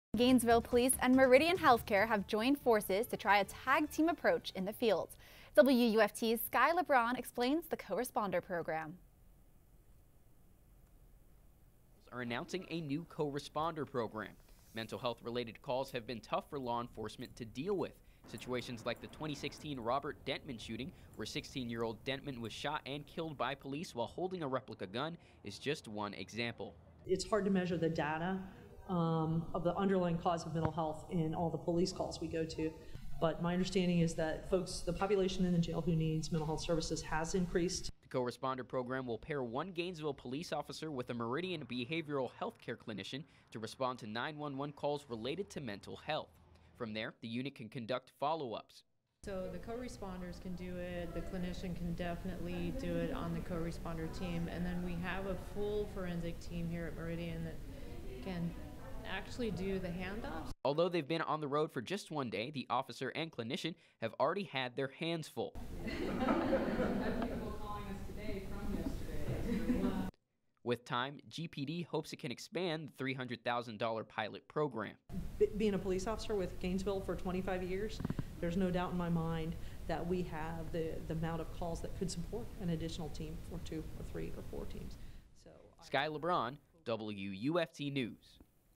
Gainesville PD Gets Co-Responders
Gainesville, FL, has a brand new police-embedded mental health clinician who is already so busy that there is talk of adding more clinicians.